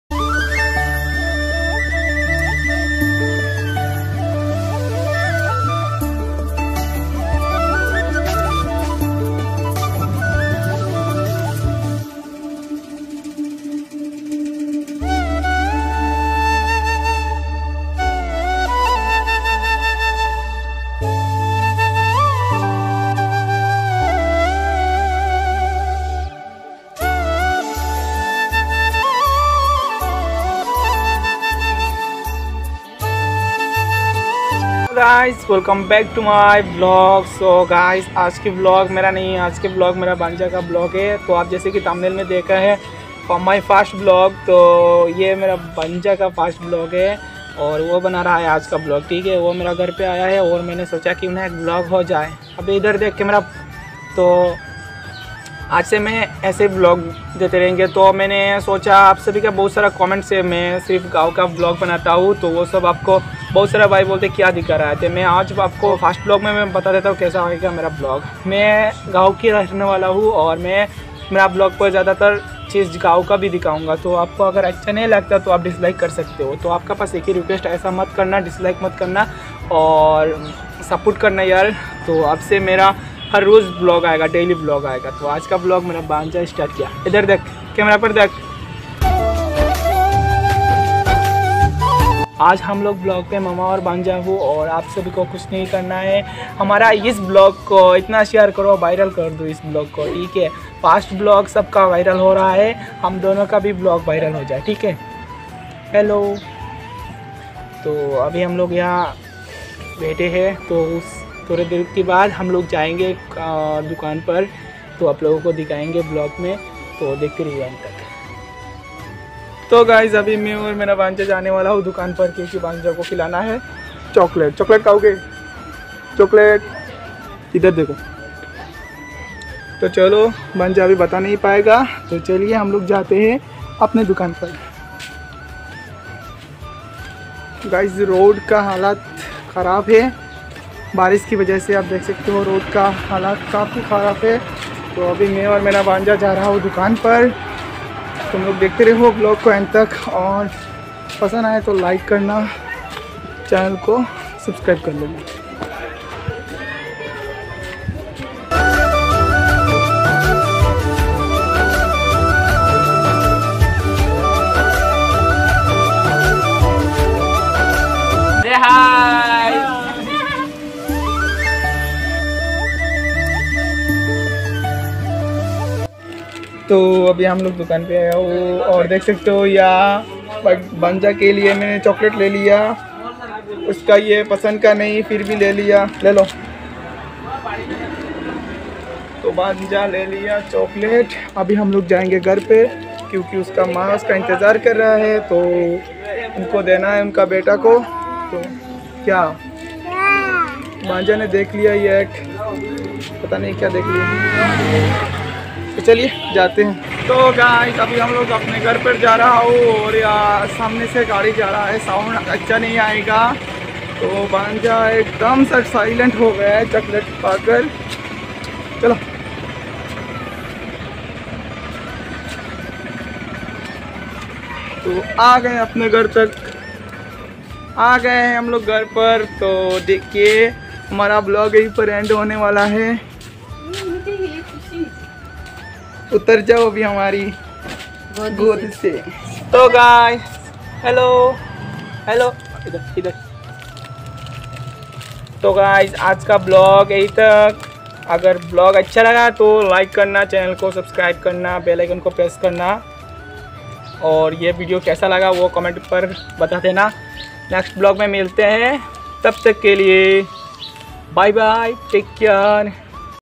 My First Vlog!! ❤️🔥
Guys Eya Mera Banja Ka First Vlog☺️kaisa Laga Comment Pe Batana Jorur, Guys Mein Gaw Mein Rehta Hu Or Mera Video Mein Jada tor gawki Ciz Dekne ko milaga aap sobiko..❤️Bs Aap Sab Peyar Dete roho, Aap Logoka peyar or support ka bohut jorurat hai. please yaar support korna.

first vlog,my first vlog 2022
vlogs
Ginuk Vlogs